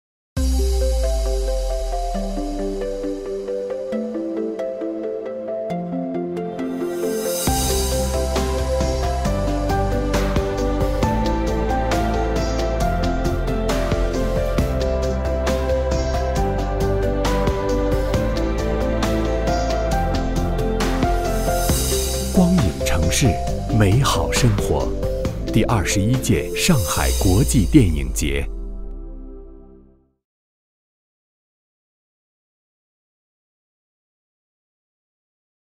Official promo video for 21st Shanghai International Film Festival released
The official promo video of the 21st Shanghai International Film Festival has been released. The Golden Goblet Awards will be published on June 16-25.